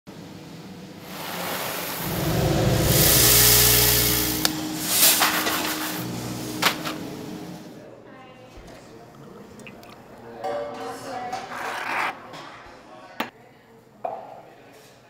Saucony Irish Coffee Pack Video 3 of 3
In collaboration with Coffee by Design, Momentum presents Saucony's Irish Coffee Pack. This is video 3 of 3.